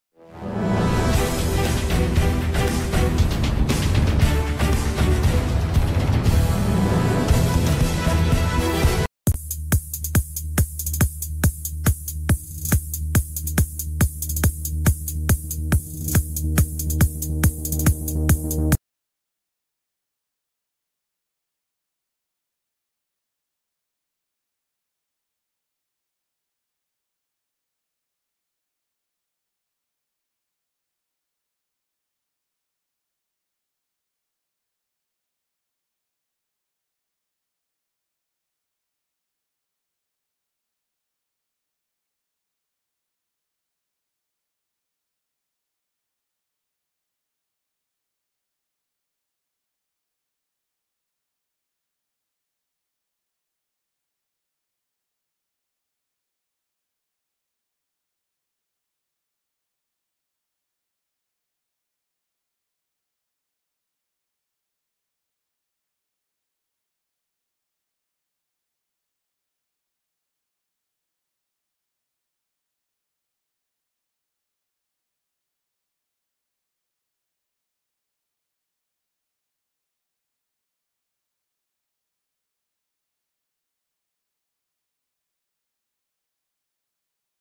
Brooklyn Nine-Nine annulée par la FOX, NBC lui offre une saison 6 !
Welcome!